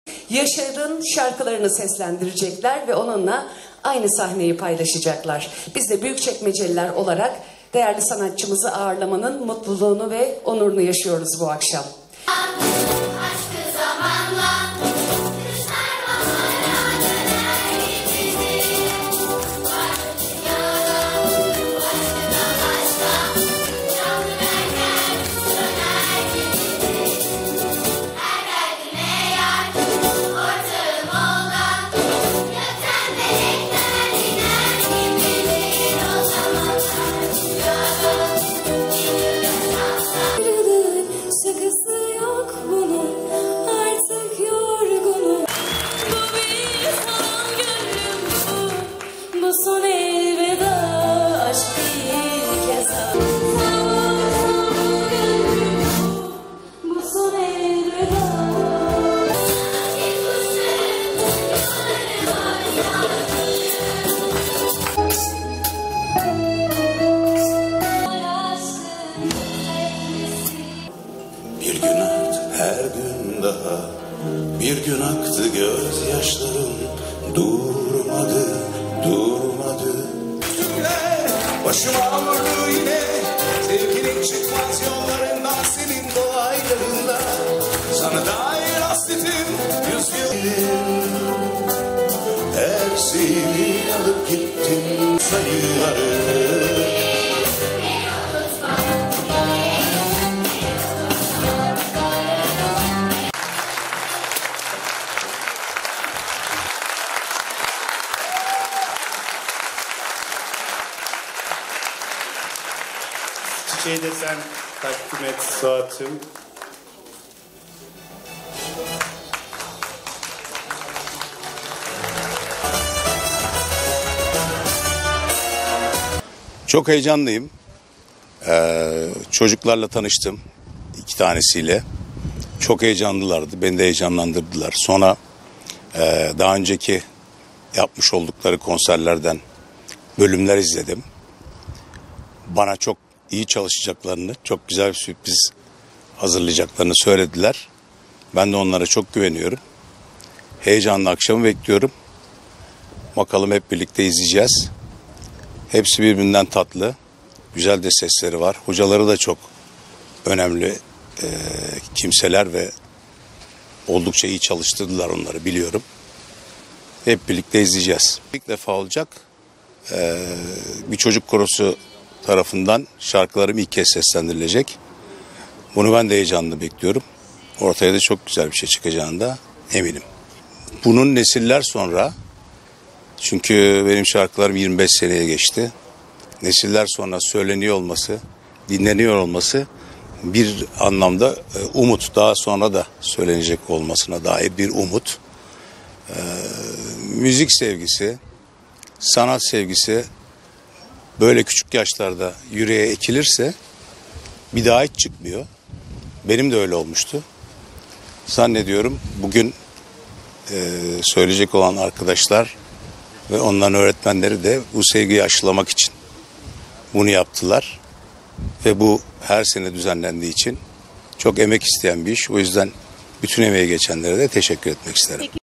Ustalara Saygı Gecesi'nde Yaşar sahne aldı
Büyükçekmece Belediyesi’nce geleneksel olarak düzenlenen 'Ustalara Saygı Gecesi'nin konuğu sevilen şarkıcı Yaşar oldu. Gecede, 'Bir Yıldız Işığında Çocuk Sesleri Korosu'nun minikleri Yaşar için sahne aldı. Kendi şarkılarını miniklerden dinleyen Yaşar, duygu dolu anlar yaşadı.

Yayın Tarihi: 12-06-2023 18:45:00